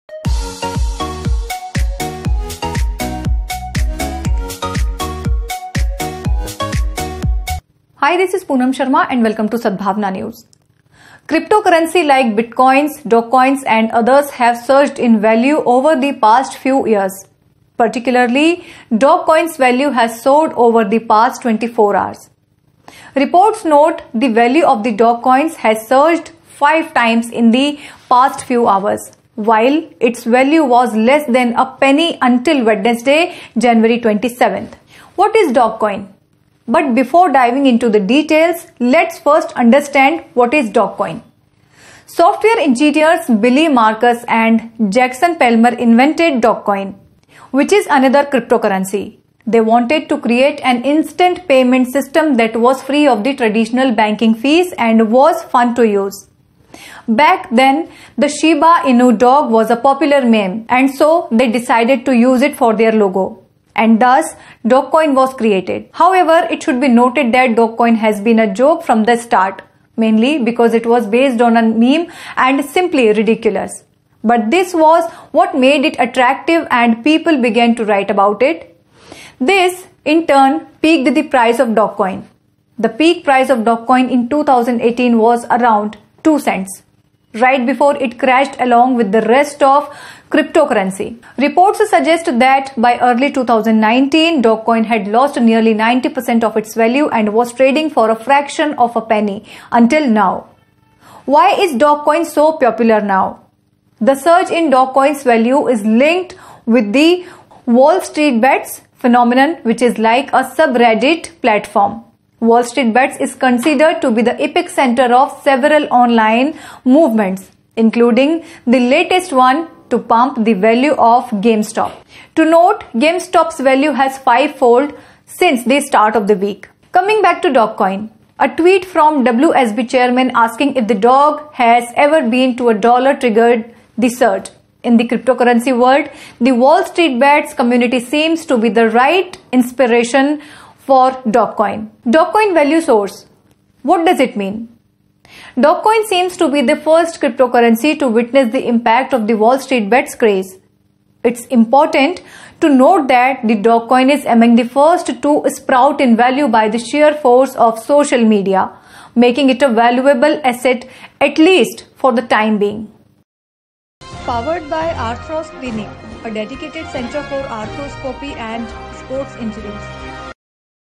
Dogecoin Value Soars What Is Dogecoin And Why Is It So Popular Right Now
A Win For Dogecoin Despite Price Pump and Dump - SuperCryptoNews
Cryptocurrency like bitcoins, dogecoins, and others have surged in value over the past few years. Particularly, Dogecoin's value has soared over the past 24 hours. Reports note the value of the dogecoin has surged five times in the past few hours, while its value was less than a penny until Wednesday (January 27).

What Is Dogecoin? But before diving into the details, let's first understand what is dogecoin. Software engineers Billy Markus and Jackson Palmer invented Dogecoin, which is another cryptocurrency. They wanted to create an instant payments system that was free of the traditional banking fees and was fun to use. Back then, the Shiba Inu dog was a popular meme, and so they decided to use it for their logo. And thus, Dogecoin was created. However, it should be noted that Dogecoin has been a joke from the start, mainly because it was based on a meme and simply ridiculous. But this was what made it attractive and people began to write about it. This, in turn, peaked the price of Dogecoin. The peak price of Dogecoin in 2018 was around two-cents, right before it crashed along with the rest of cryptocurrency. Reports suggest that by early 2019, Dogecoin had lost nearly 90 percent of its value and was trading for a fraction of a penny, until now! Why Is Dogecoin So Popular Now? The surge in Dogecoin's value is linked with the WallStreetBets phenomenon, which is like a sub-Reddit platform. WallStreetBets is considered to be the epic center of several online movements, including the latest one to pump the value of GameStop. To note, GameStop's value has fivefold since the start of the week.

Coming back to Dogecoin, a tweet from WSB Chairman asking if the Doge has ever been to a dollar triggered the surge. In the cryptocurrency world, the WallStreetBets community seems to be the right inspiration for Dogecoin.

Dogecoin Value Soars: What Does It Mean? Dogecoin seems to be the first cryptocurrency to witness the impact of the WallStreetBets craze. It's important to note that Dogecoin is among the first to sprout in value by the sheer force of social media, making it a valuable asset - at least for the time being.